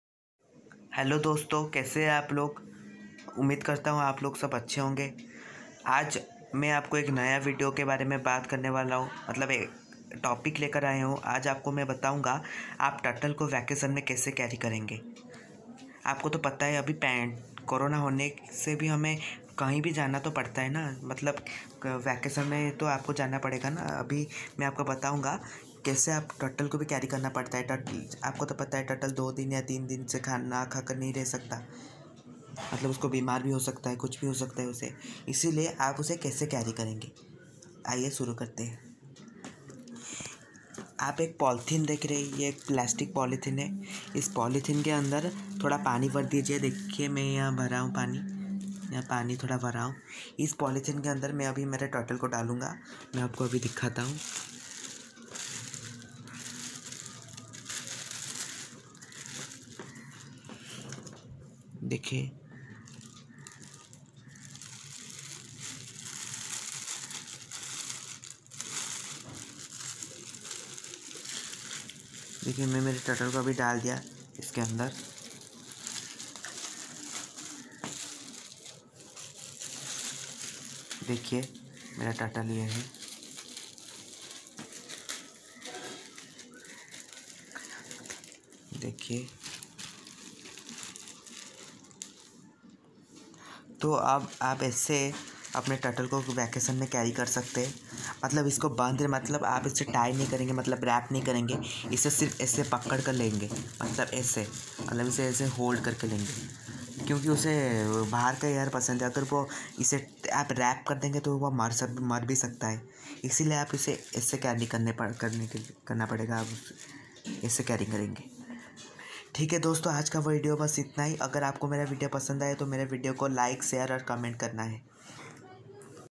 How to carry your turtle during vacation
App turtle ko vacation me kese transport karenge iske upar me aaj video banaya hon. Please watch this video 👍❤